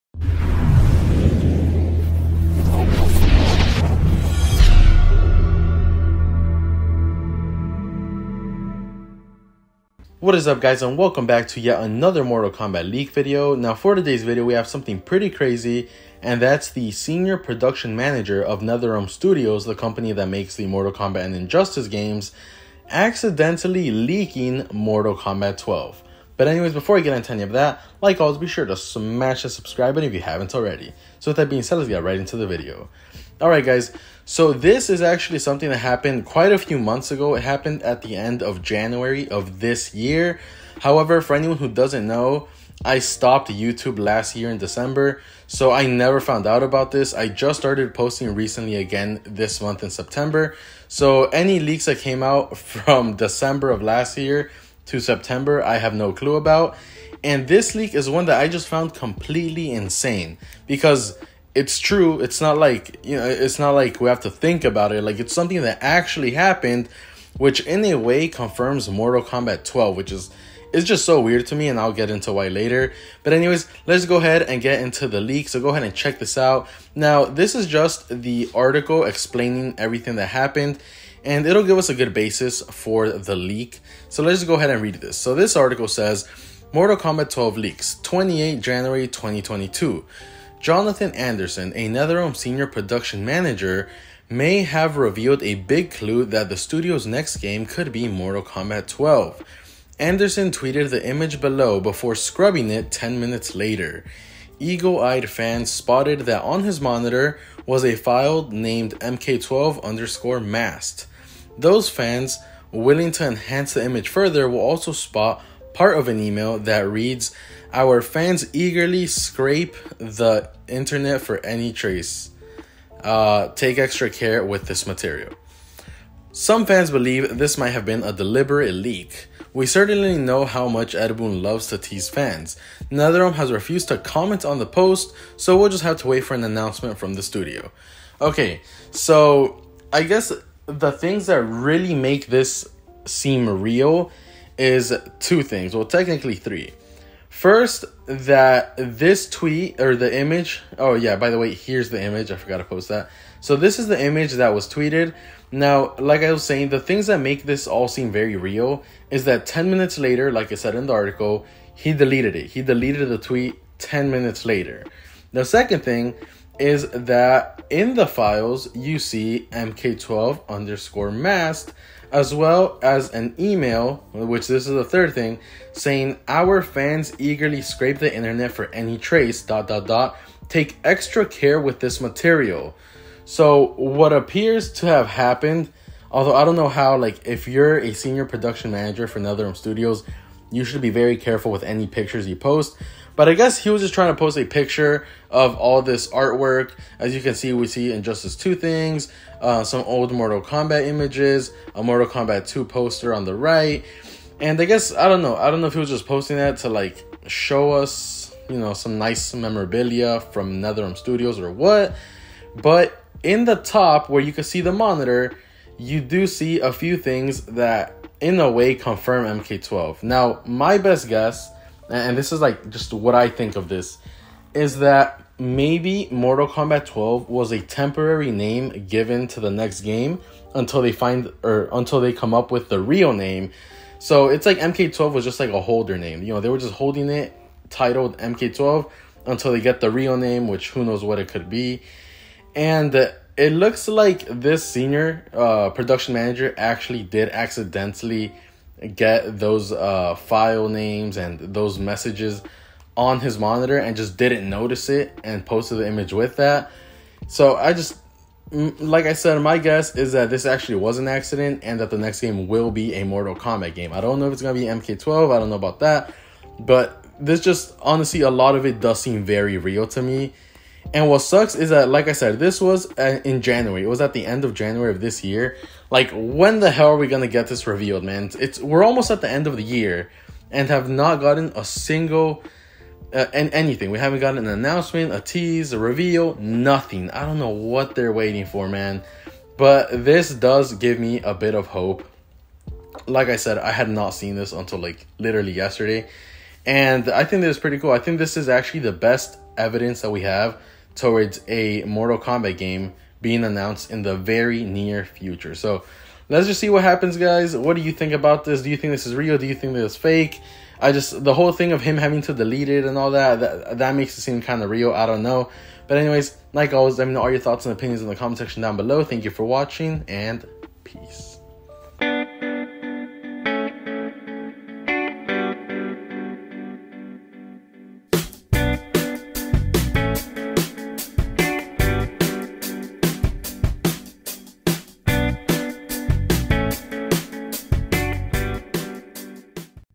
Mortal Kombat 12 Leaked by NRS Employee on Twitter!
Can't believe this actually happened but wow, I guess Mortal Kombat will indeed be our next game!